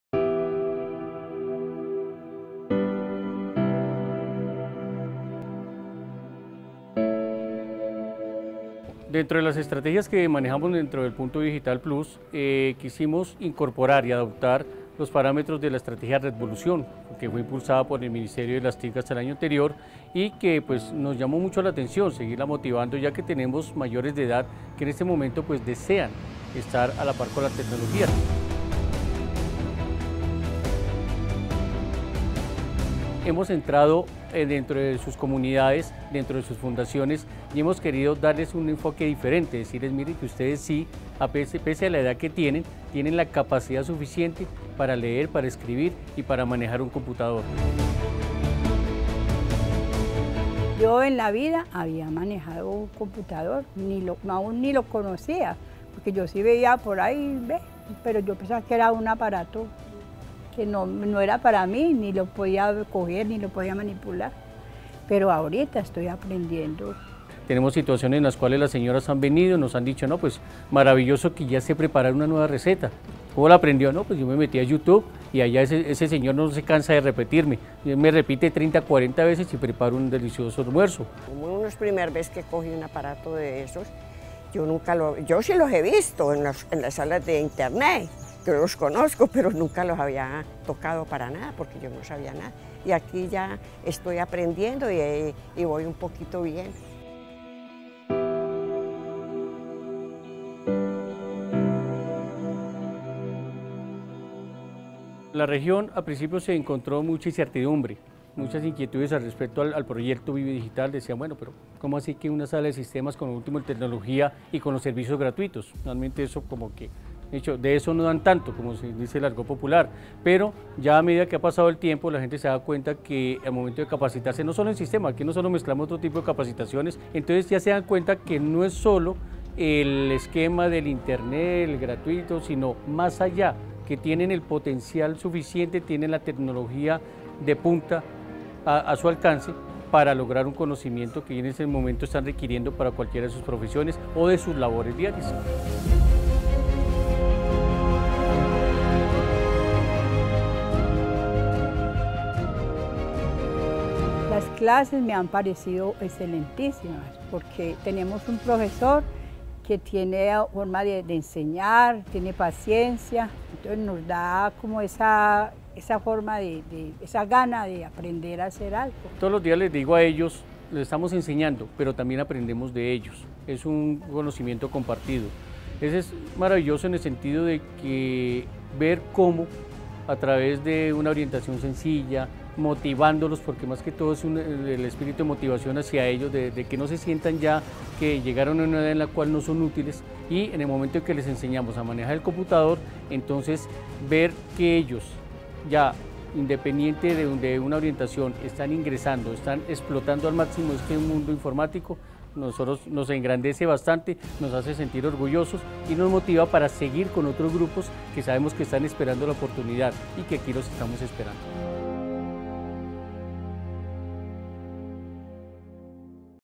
Adultos mayores aprenden en los Puntos ViveDigital Plus C34 N7
La edad no es impedimento para seguir aprendiendo, y es lo que este grupo de adultos mayores demuestra al aprender y apropiarse de las TIC en los Puntos ViveDigital Plus. Conoce más de su historia en ViveDigitalTV